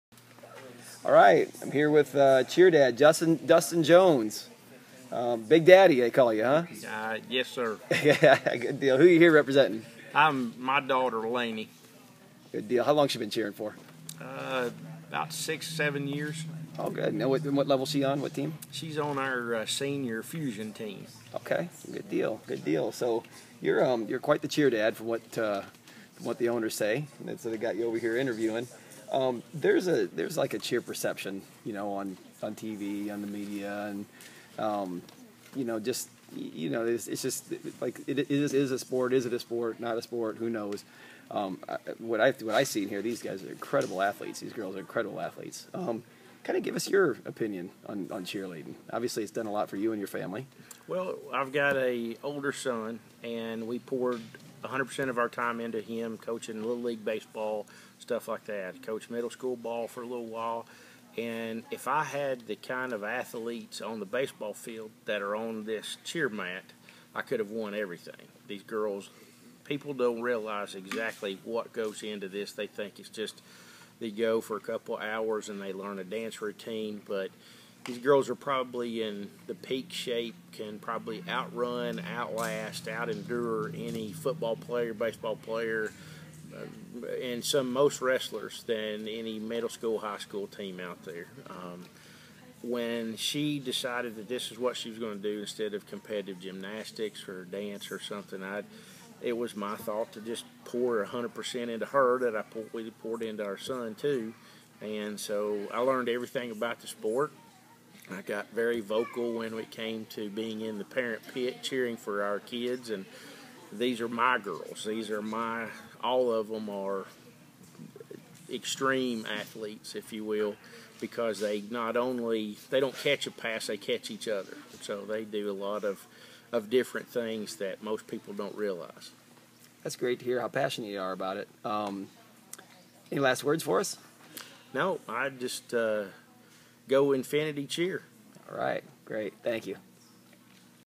Cheer dad Dustin "Big Daddy" Jones interview
We recently got a chance to speak to and visit the Infinity All Stars Gym, Owners, Coaches & Parents. This is a great cheer organization that is doing some  awesome things in and for the world of cheer. Their Facility is located at 7675 N Lee Hwy
Cleveland, TN. Check them out.